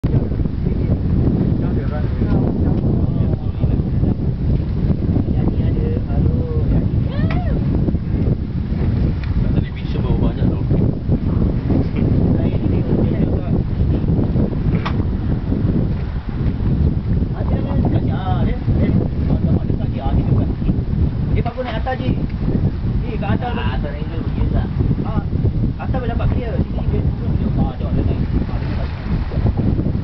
Dolphins
Dolphins in Singapore offshore island, Raffles Lighthouse January 2005.